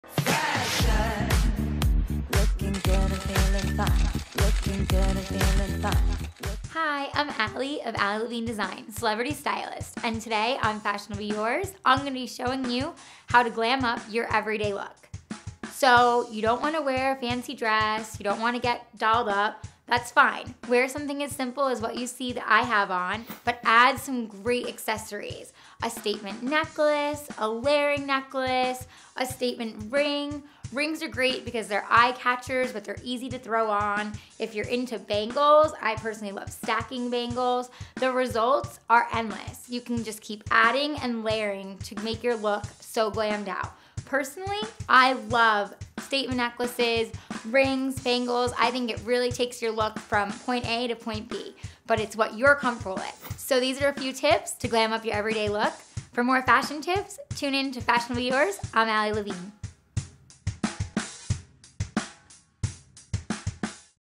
How to Glam Up Your Everyday Look
Hey Dolls,
Want to be comfortable but not look like a bum? Watch to see how to glam up your everyday   look with just a few key details and accessories!

Fashionably yours,
Ali Levine